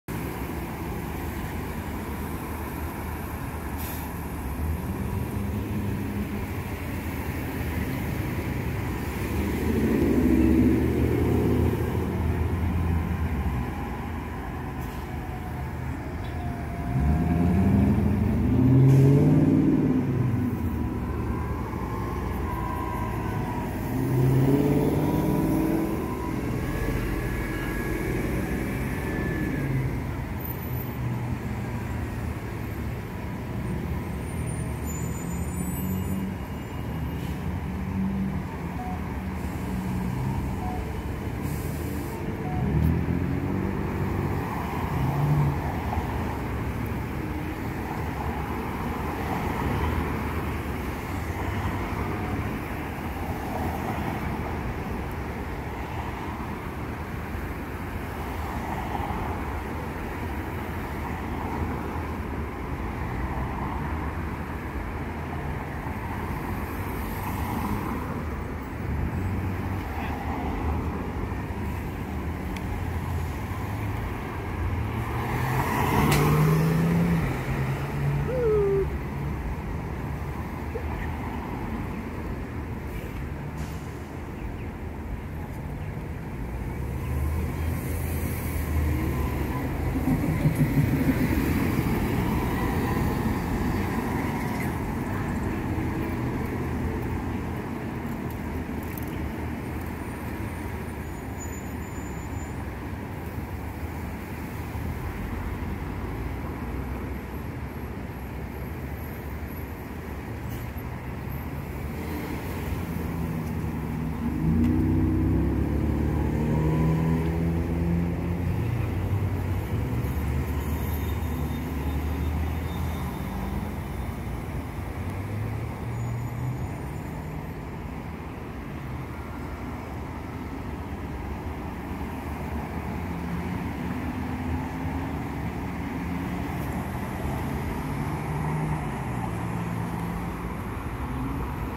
LED & Incandescent 3M Traffic Lights (University Ave & 6th Ave)
This is an older video from October 2020 and I just re-premired it with the rest of the signals here. There has been some changes made since then and it's the 3M yellow arrow is LED and so is the solid red light. In this video you see they have incandescent bulbs.

Heres my newer video of the 3M signals later on when they had the new LED's in them. Also you will see an ambulance trigger the traffic lights.

The red lights have 15 LED's and 1 Incandescent
All 8 pedestrian signals are LED hand/man